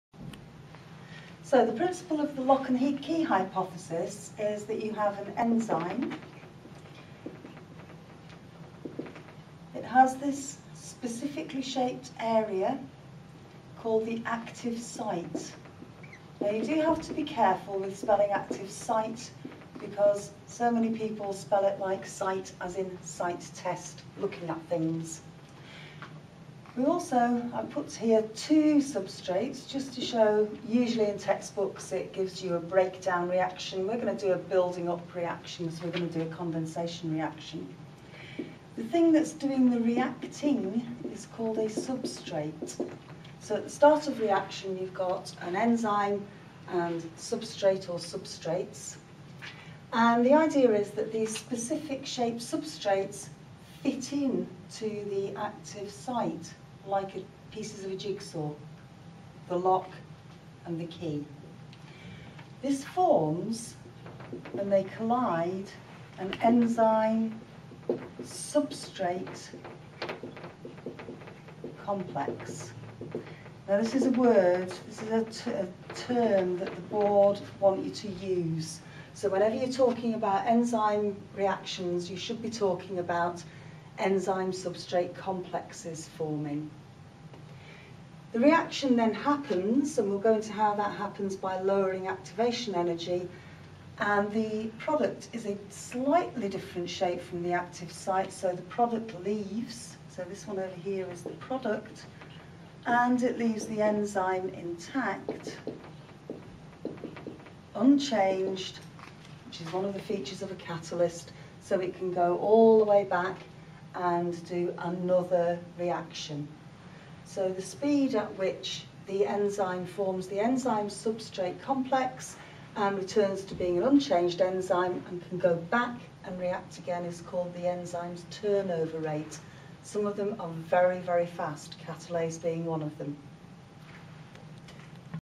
Enzymes 2 How they work
The Holy Cross Biology Team introduce you to how enzymes work